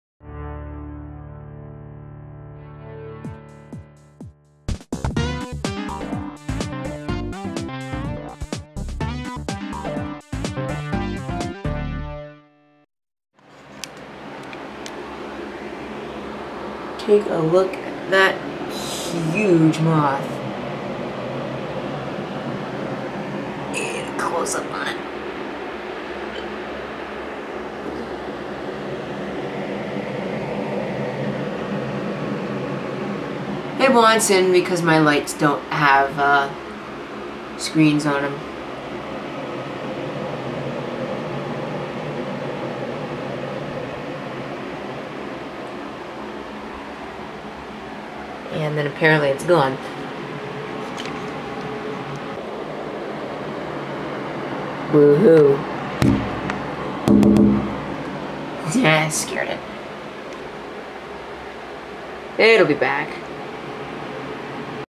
Huge Moth
Just what the title says. It seems that it's missing part of it's tail as well, but I don't know for sure.